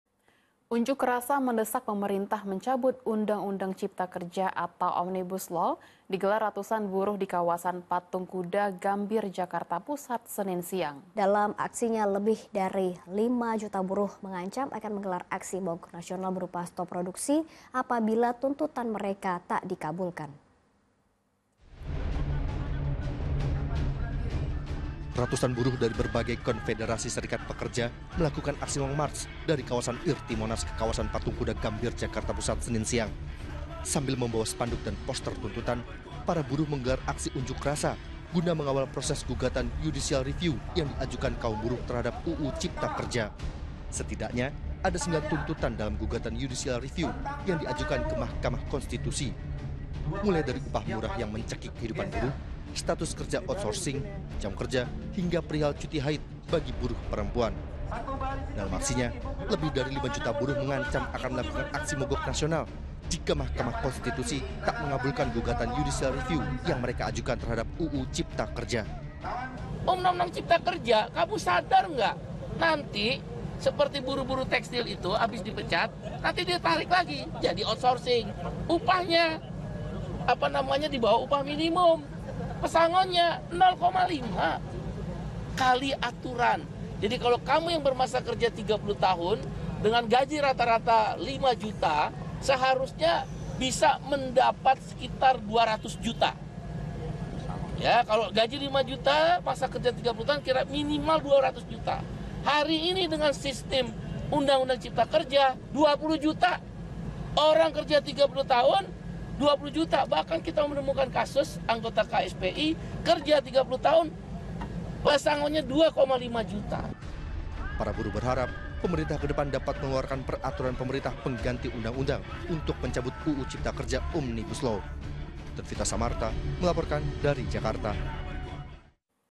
Ratusan Buruh Demo Desak Pemerintah Cabut UU Cipta Kerja, Ancam Akan Mogok Nasional | Fokus
Unjuk rasa mendesak pemerintah mencabut Undang Undang Cipta Kerja atau Omnibus Law digelar ratusan buruh di kawasan Patung Kuda, Gambir, Jakarta Pusat, Senin (08/07) siang. Dalam aksinya, lebih dari 5 juta buruh mengancam akan menggelar aksi mogok nasional berupa stop produksi apabila tuntutan mereka tak dikabulkan.